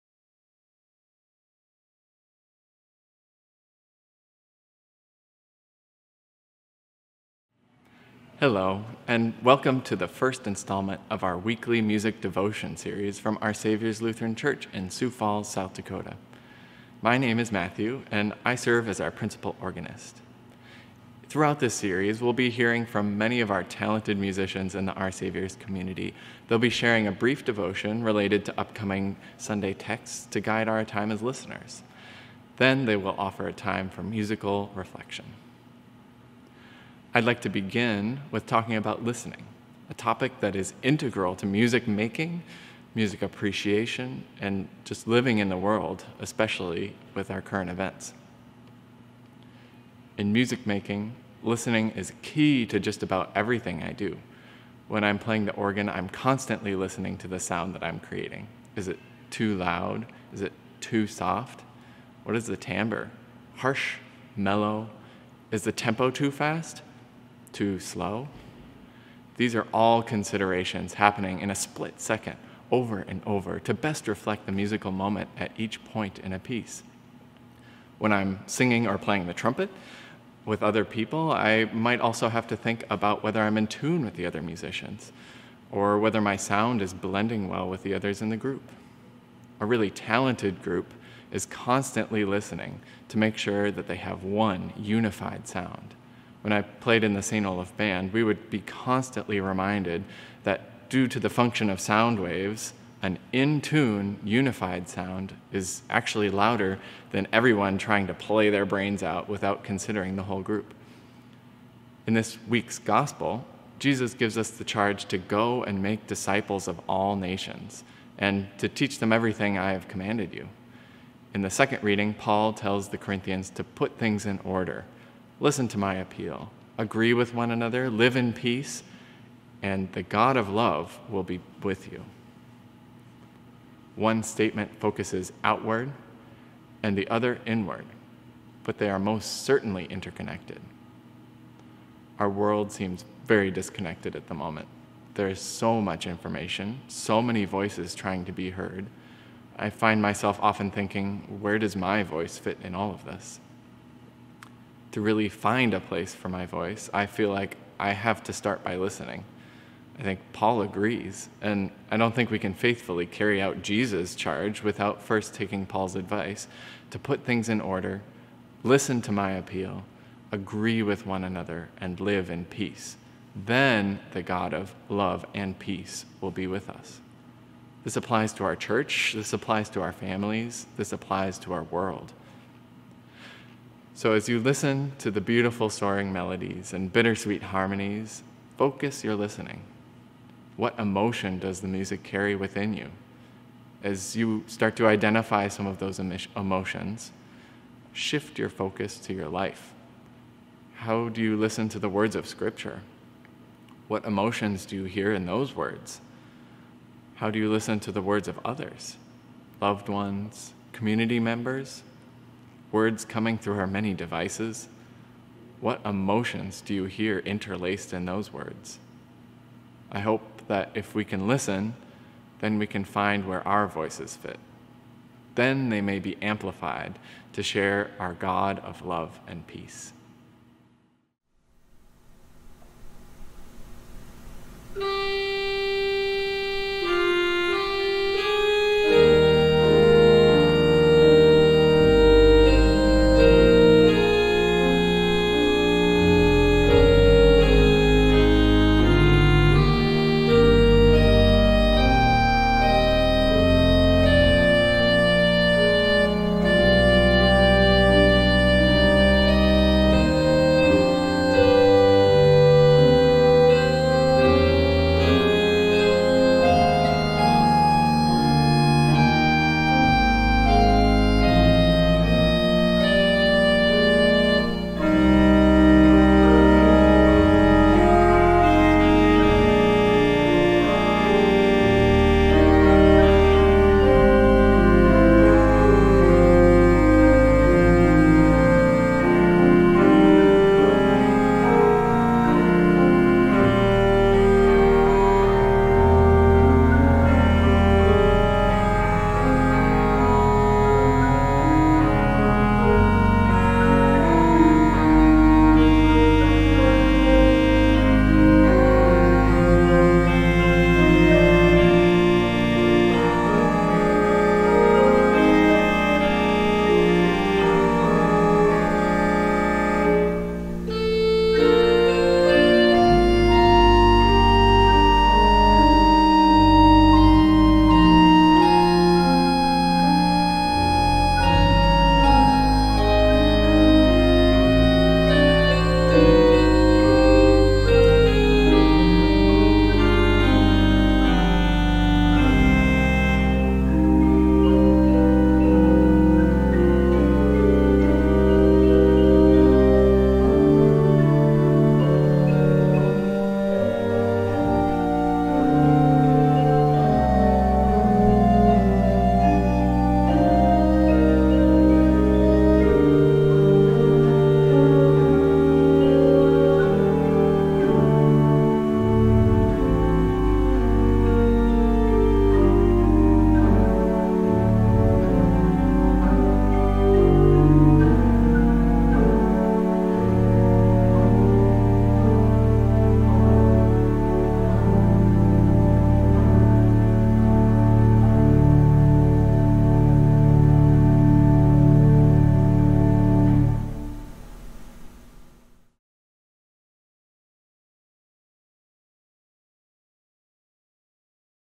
OSL Weekly Music Devotion Series, Ep 1
OSL’s Weekly Music Devotion Series features talented musicians from the OSL community sharing a brief devotion related to the upcoming Sunday texts, followed by a musical reflection. In this first episode, we hear from Matthew Tylutki, OSL’s Principal Organist.